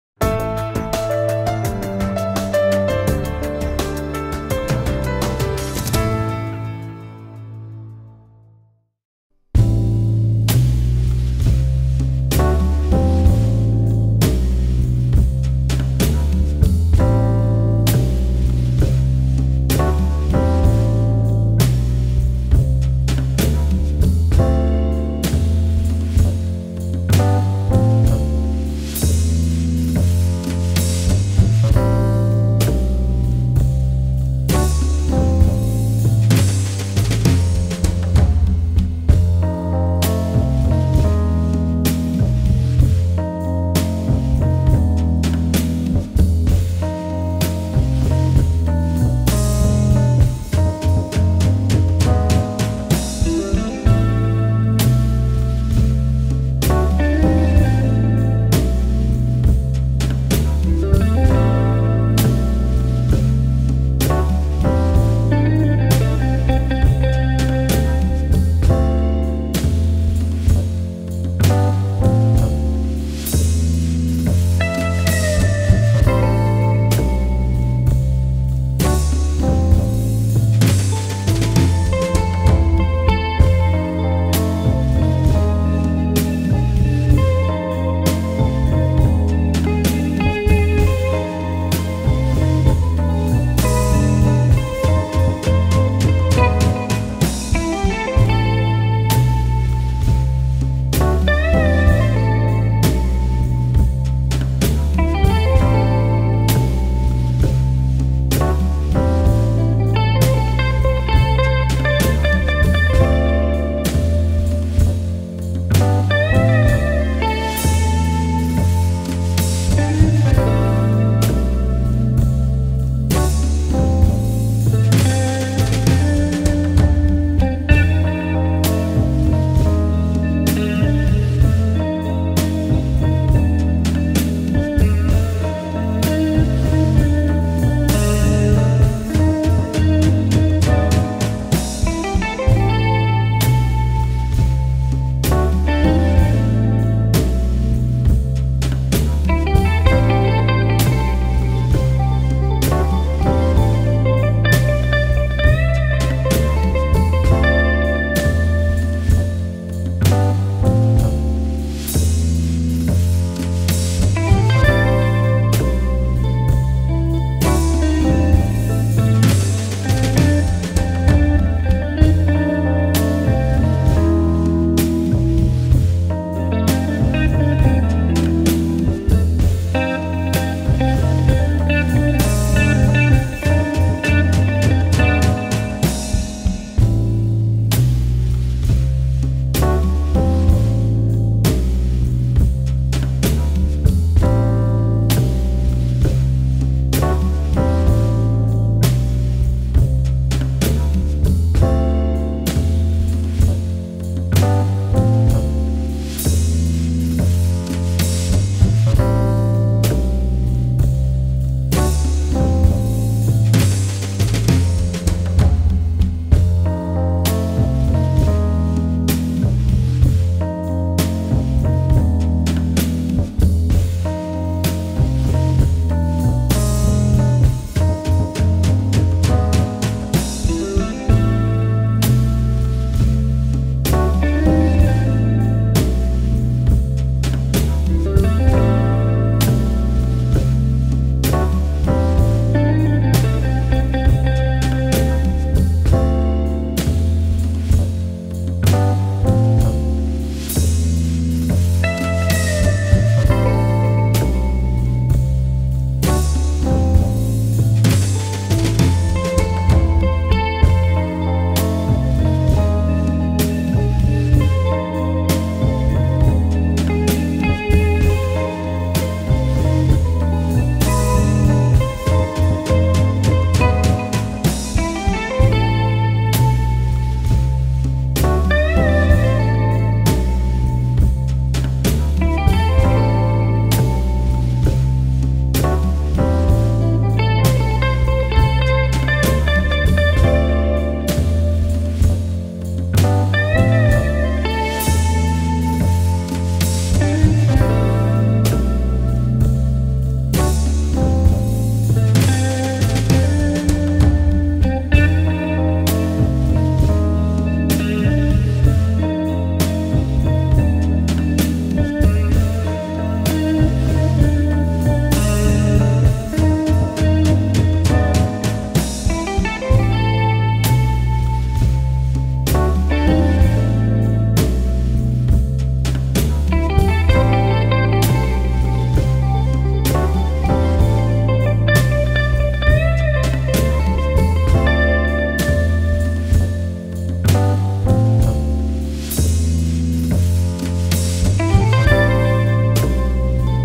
Сухая кисть и карандаш
Не судите строго, я не профессионал, а только любитель... К сожалению пришлось выложить это видео повторно, так как были утеряны логин и пароль предыдущего аккаунта. Но нет худа без добра - в этом видео есть и новые работы)))